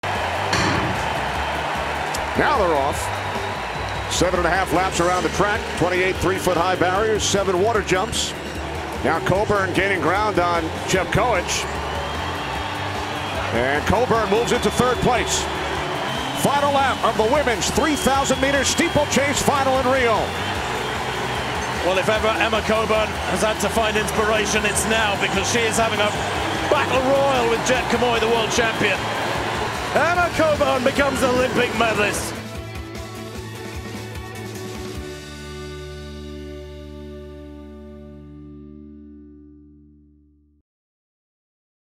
Emma Coburn Becomes First USA Woman To Medal In Steeplechase
Emma Coburn won the bronze medal in the 3,000-meter steeplechase to bring home the first olympic medal for Team USA in the competition at the Olympic Games Rio 2016.